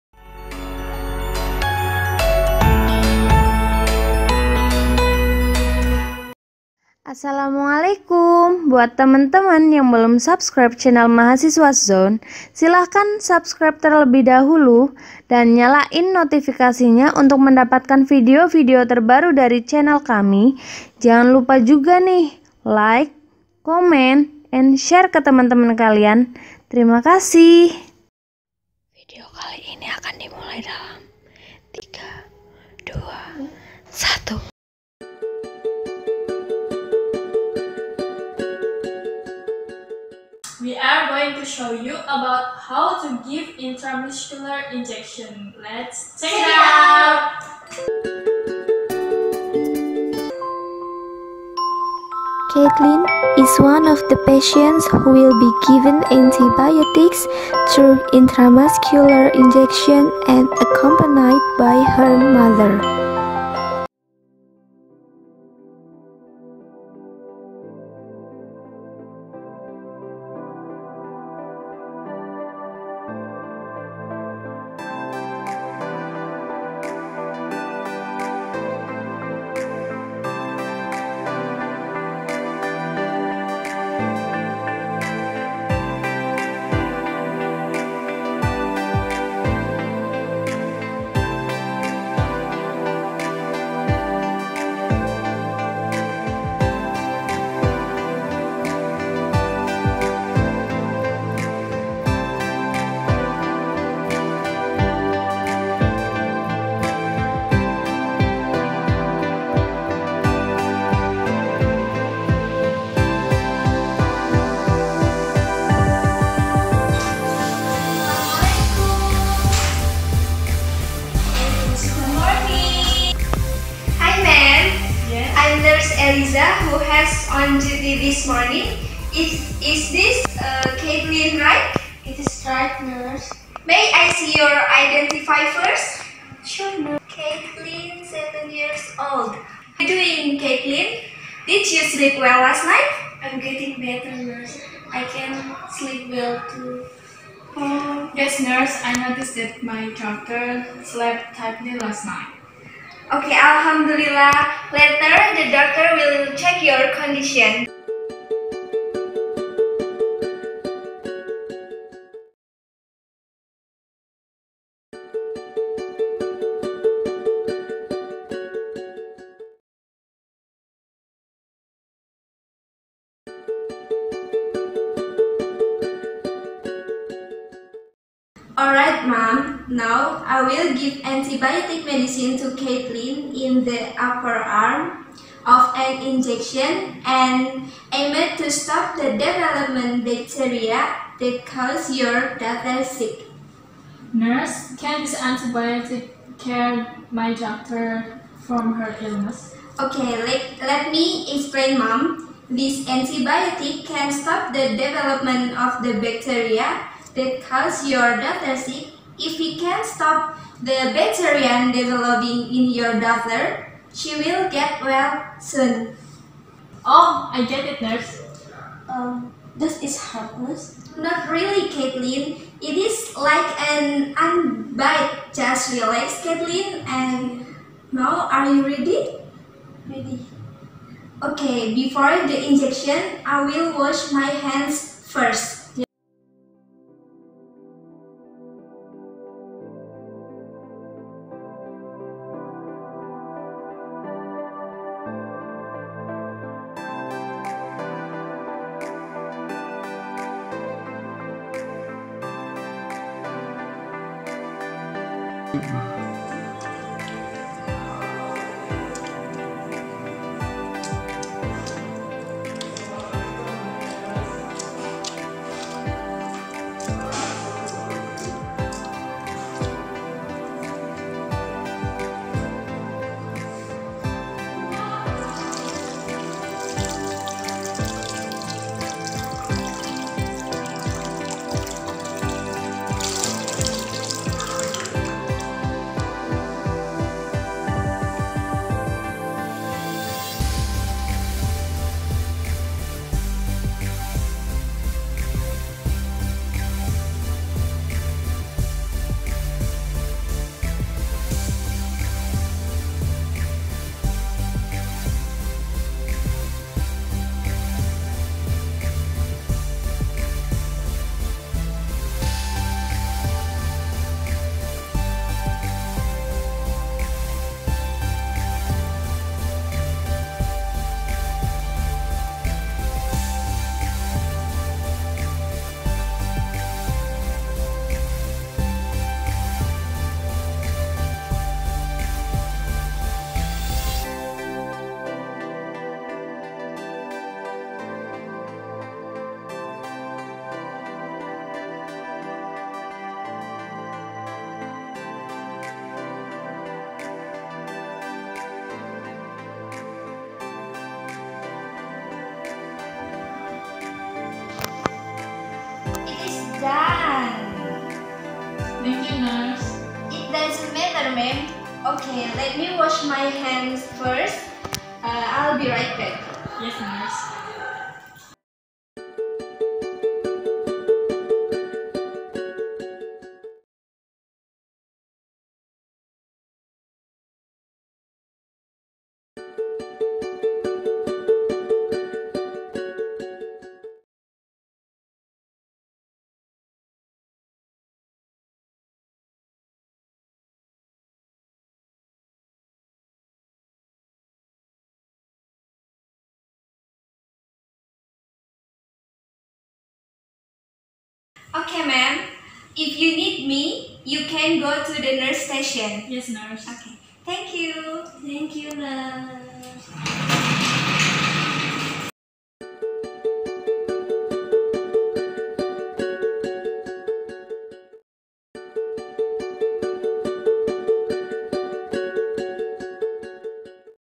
INJEKSI INTRAMUSKULAR (IM) I INTRAMUSCULAR INJECTION I CARA INJEKSI IM
Video ini berisi tentang cara-cara melakukan injeksi intramuskular
diawali dari tahap orientasi, kerja, lalu terminasi